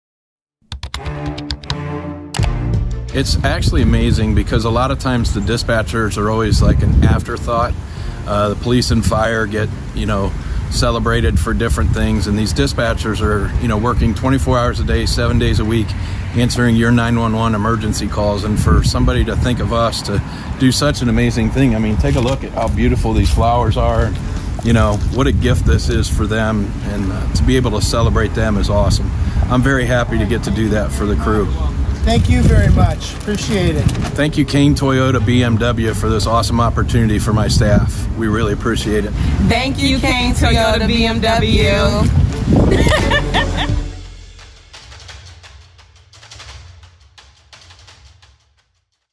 Dispatcher Appreciation at Cain BMW
We wanted to show our appreciation for the dispatchers in our area who work tirelessly day in and day out so we hired Graf Growers flower truck to brighten up their day.
6461 Whipple Ave Nw
North Canton, Ohio 44720